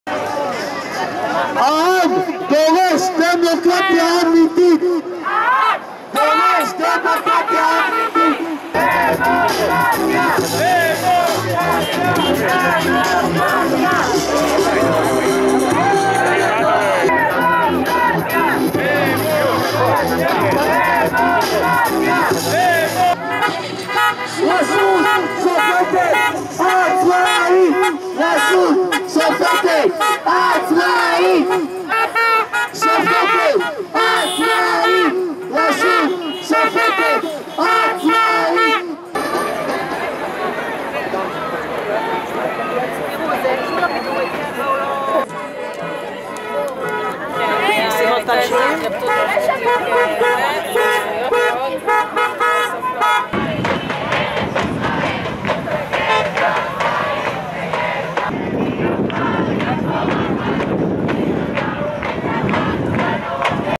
טבעון נגד הפיכה משטרית 13.2.2023 Kiryat Tiv'on against a coup d'état
ענת ג'ורג'י: ברגע הראשון הכל ייראה אותו דבר, ואז לאט־לאט החיים שהכרנו  ישתנו.
מתוך מגילת העצמאות:
מדינת ישראל תהא פתוחה לעליה יהודית ולקיבוץ גלויות; תשקוד על פיתוח הארץ לטובת כל תושביה; תהא מושתתה על יסודות החירות, הצדק והשלום לאור חזונם של נביאי ישראל; תקיים שויון זכויו​ת חברתי ומדיני גמור לכל אזרחיה בלי הבדל דת, גזע ומין; תבטיח חופש דת, מצפון, לשון, חינוך ותרבות; תשמור על המקומות הקדושים של כל הדתות; ותהיה נאמנה לעקרונותיה של מגילת האומות המאוחדות. מדינת ישראל תהא מוכנה לשתף פעולה עם המוסדות והנציגים של האומות המאוחדות בהגשמת החלטת העצרת מיום 29 בנובמבר 1947 ותפעל להקמת האחדות הכלכלית של ארץ-ישראל בשלמותה.